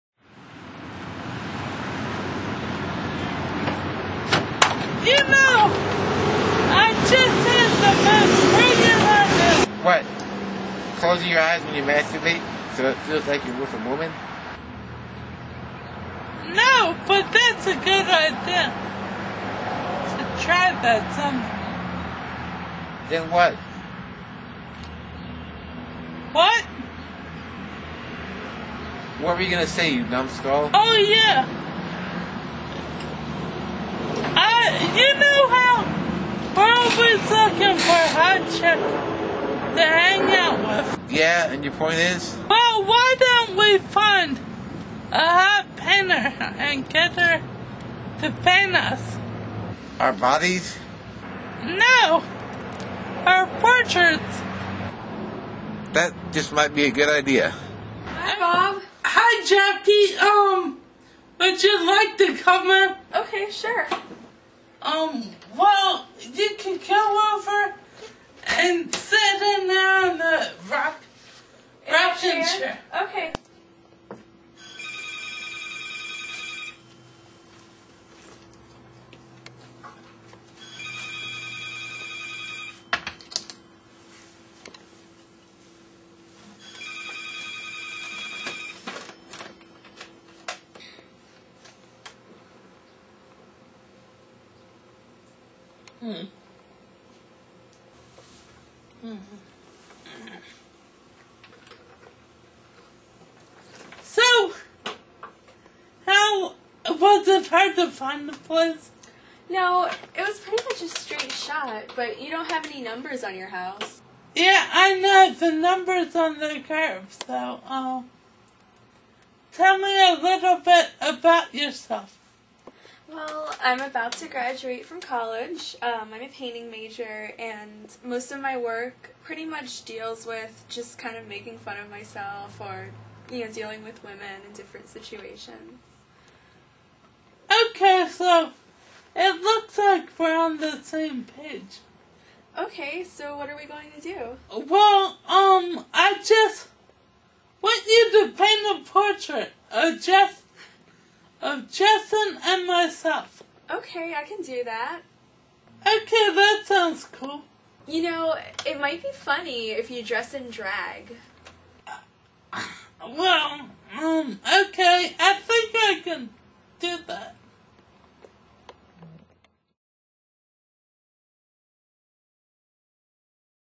Balls Painted Blue #1
Bob and Justin get a hot girl to paint them.